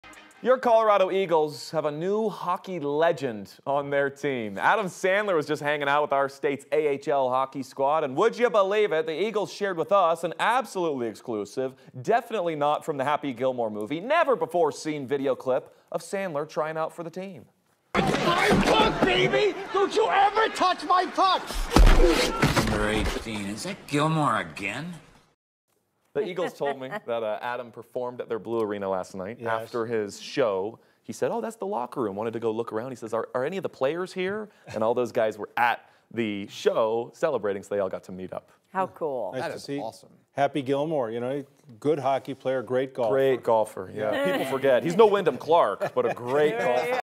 Adam Sandler takes photo with Colorado Eagles hockey team
The comedian and actor has an upcoming show at Ball Arena on Dec. 12.

9NEWS (KUSA) is located in Denver, Colorado.